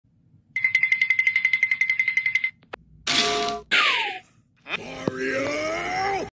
dad pranks (fathers day special)
mario tickles dads nose.